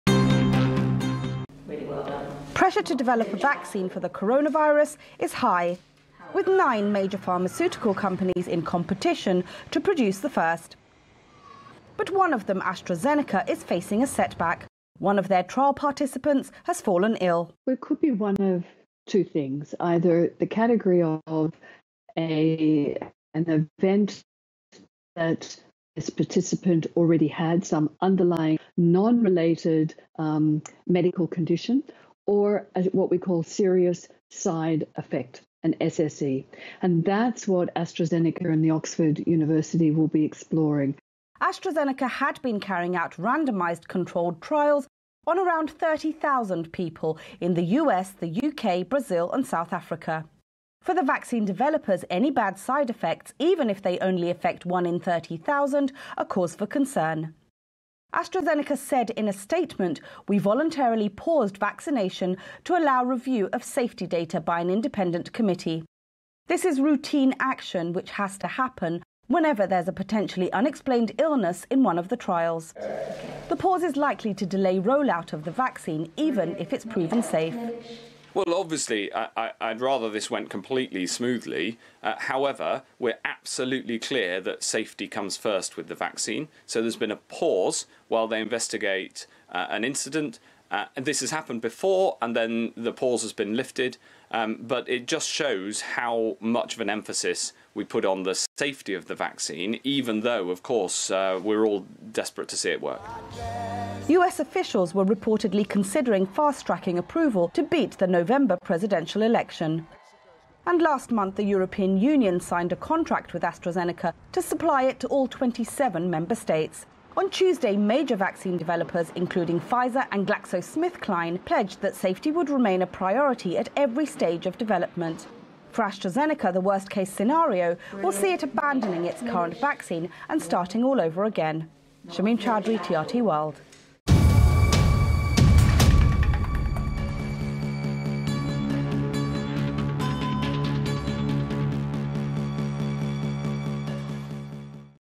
AstraZeneca suspends Covid-19 vaccine trial over safety concern
Pharmaceutical company AstraZeneca has paused the trial of its highly-anticipated Covid-19 vaccine. That's after one participant reportedly had a serious adverse reaction. The vaccine is being developed in collaboration with the University of Oxford. AstraZeneca says it is reviewing safety data. Shamim Chowdhury has the story.